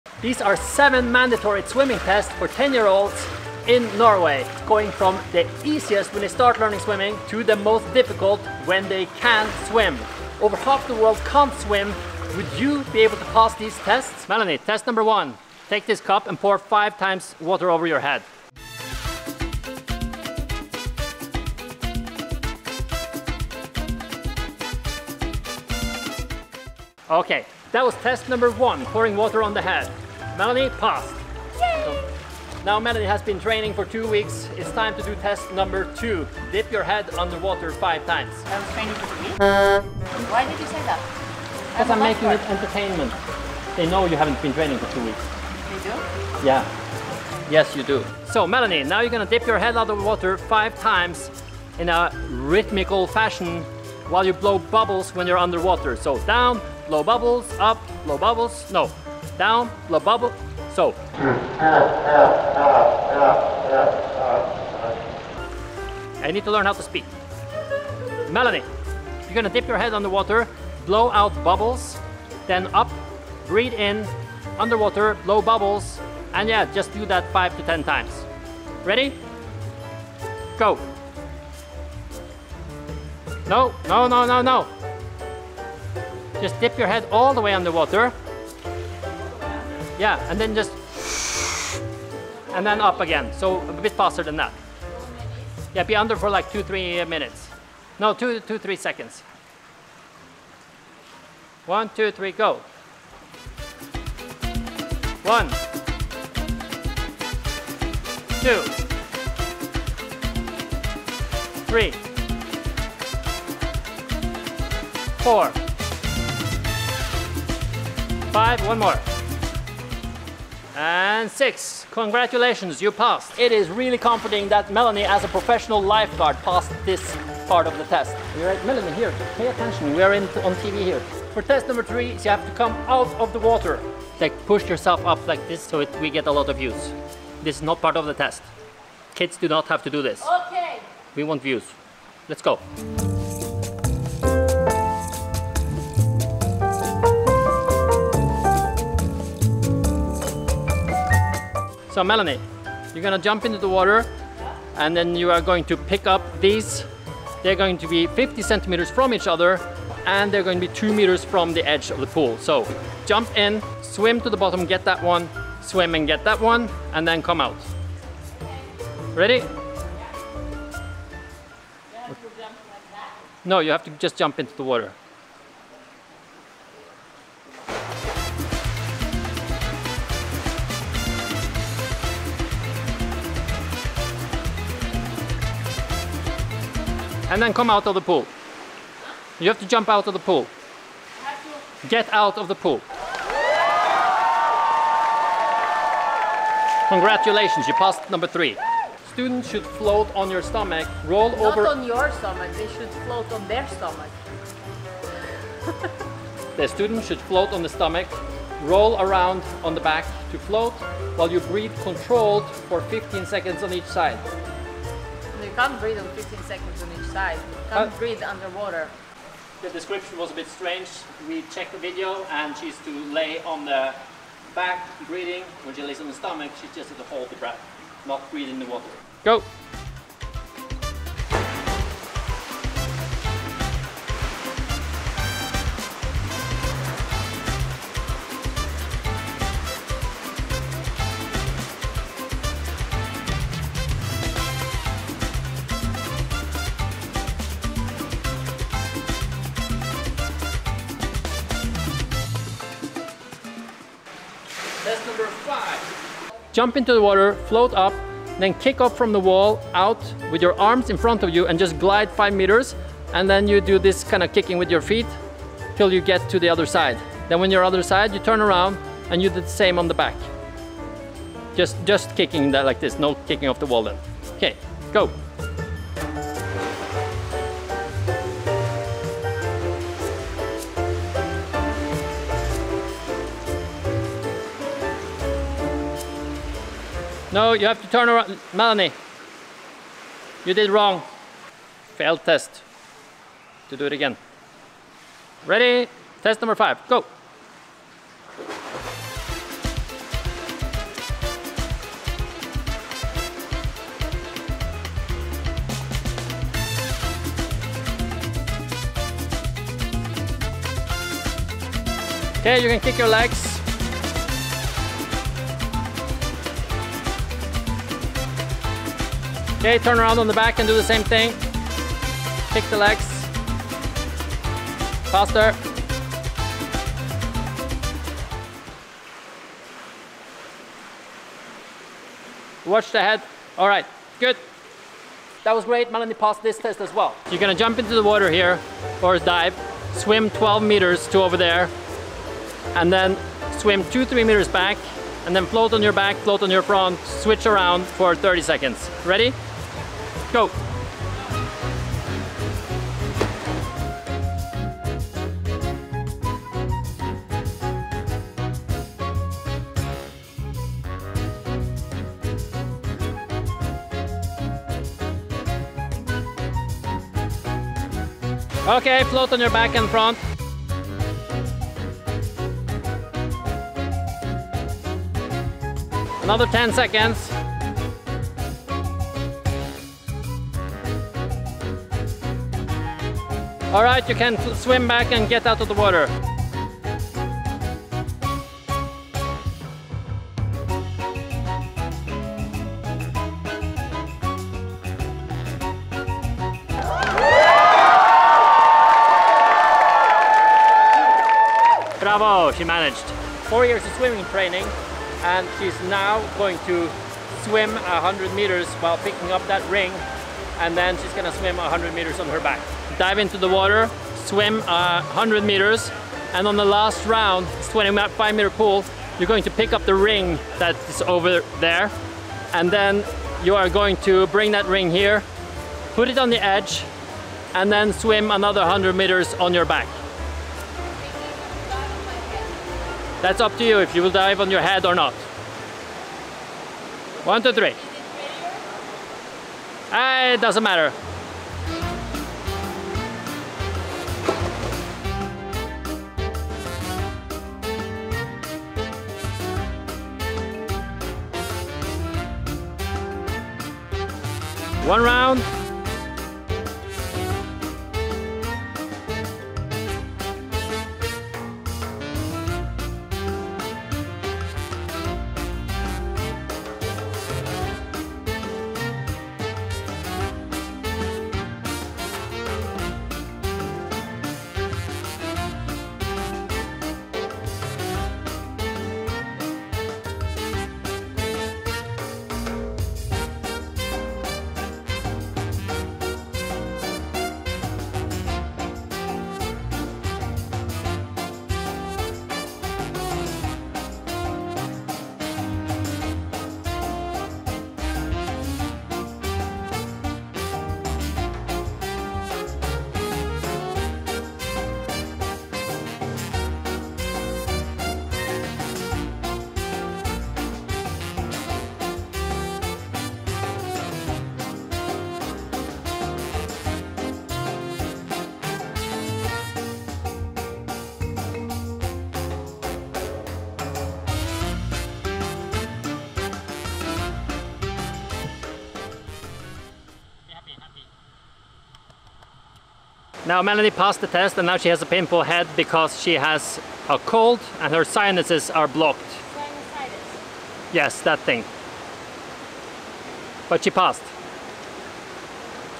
Swimming Test For 10 Year Olds - Would You Pass?
This is the mandatory swimming test for 4th graders in Norway?
Half of the world can not swim and would fail this test, would you pass it?